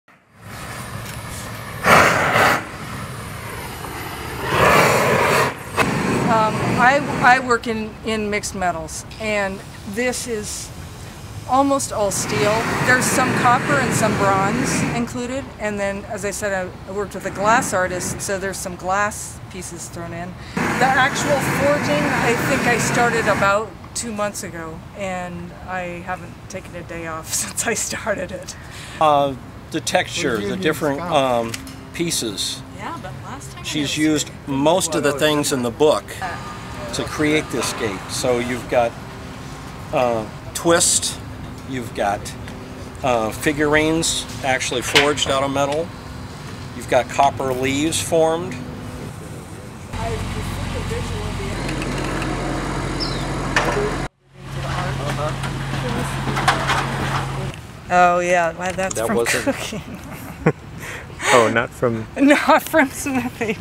Artist Heather McLarty's Newest Work at Oxy
Los Angeles metal artist Heather McLarty talks about her newest work as it is installed at Occidental College's Jack Kemp Stadium. Glass artist Cathi Milligan made all the glass elements.

Video by Marc Campos